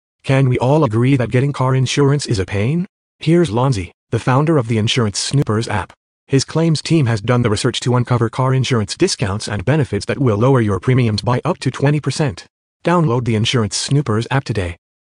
The Only Car Insurance for All Drivers: The Insurance Snoopers App!
Getting Car Insurance Is Time Consuming and Costly. Filling out multiple questionnaires and forms just to get rejected or face extremely higher premiums for 2021 is absurd and unacceptable. But getting the right help to find the best policy, discounts, and benefits is on the way!
and start saving today.